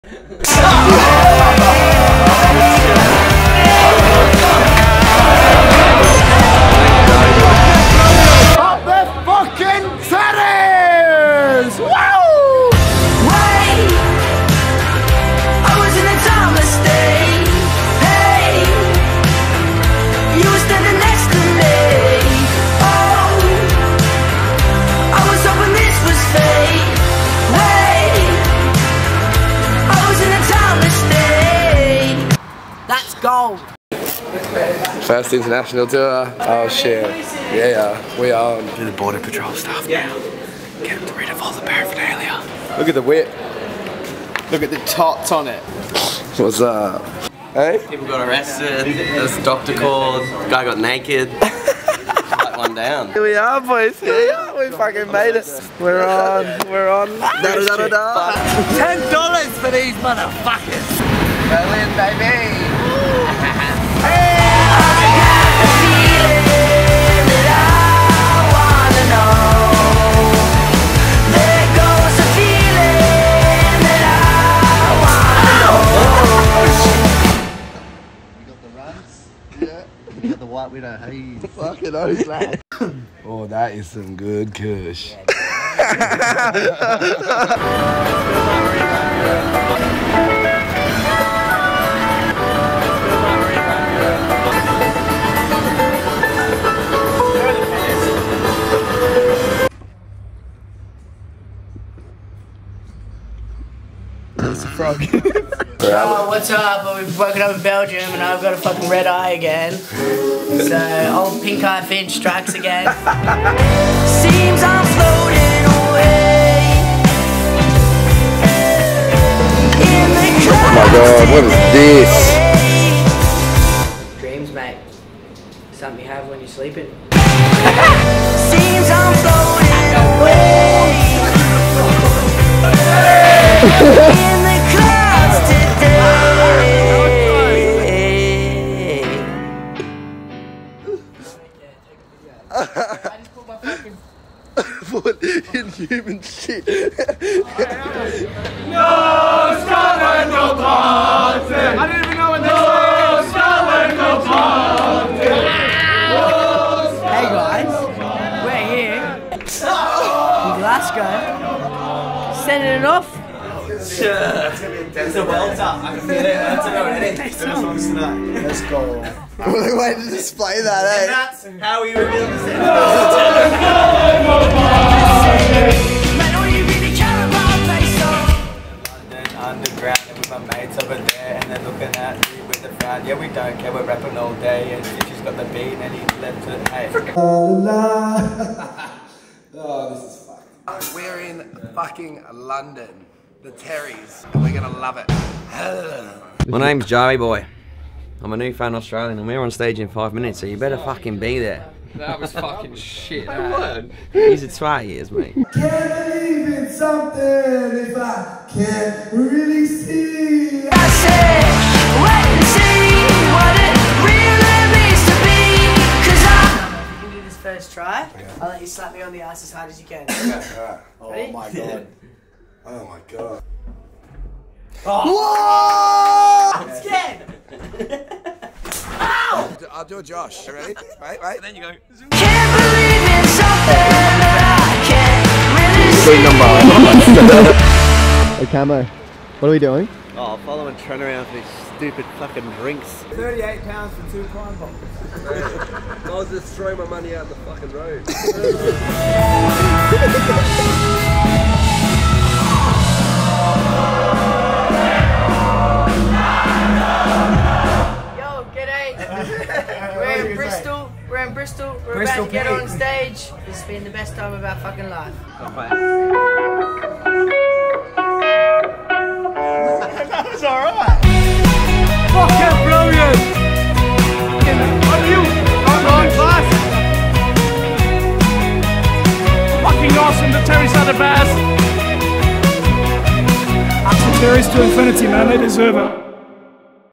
The Terrys - debut EU/UK tour aftermovie
That was the most wild, silly, humbling, financially crippling, fun, this can't be happening type shit we've ever done in our entire life. The fact we're going to be back in less then three months is crazy too.

🎥 by the one and only Josh Hickie